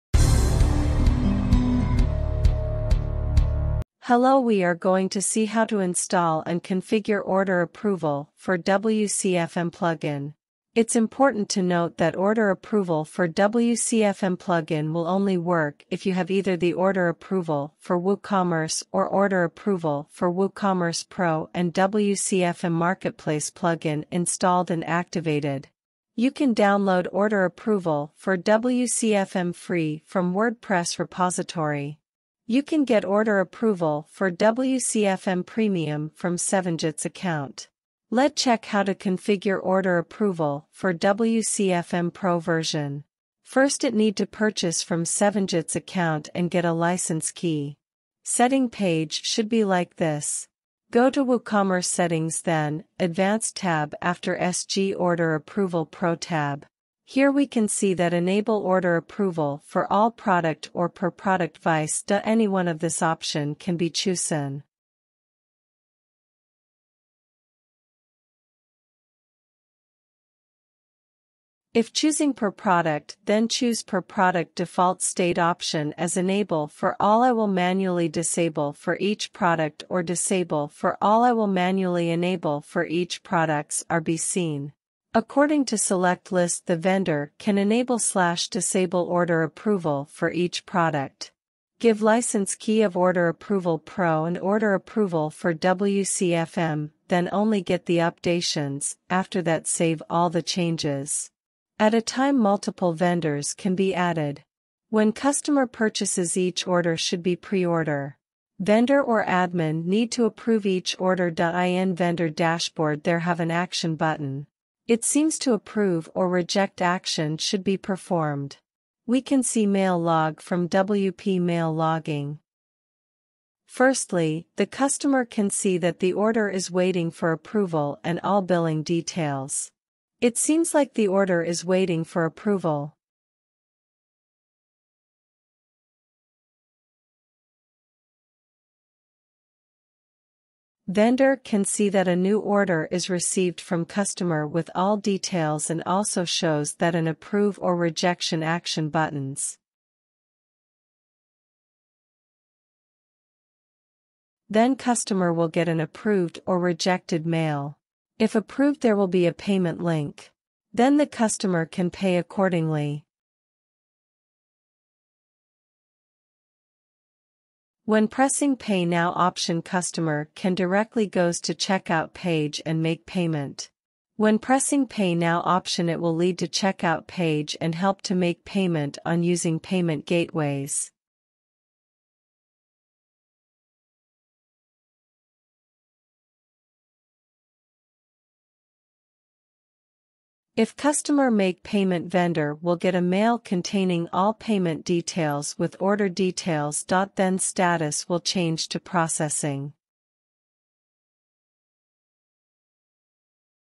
Order Approval For WCFM | Addon for Order Approval Plugin - Sevengits
Introducing the "Order Approval for WCFM" plugin, the solution you've been looking for to empower WCFM marketplace vendors. Learn how this plugin simplifies order management by allowing vendors to approve or reject customer orders before payments.

👉 Order Approval for WCFM Free Features :
✅  Improved order management
✅  Increased customer satisfaction
✅  Reduced payment issues

👉 Order Approval for WCFM Pro Features :
✅  Vendor-Centric Control
✅  Processing payments in a transparent manner only after receiving vendor consent builds trust.
✅  Integrates seamlessly and simply with the WCFM Marketplace Plugin.
✅  Customizable Approval Criteria
✅  Effective order communication
✅  Reduced Order Cancellations
✅  Vendors control the payment processing process to make sure that payments match approved orders.
✅  Comprehensive Order Management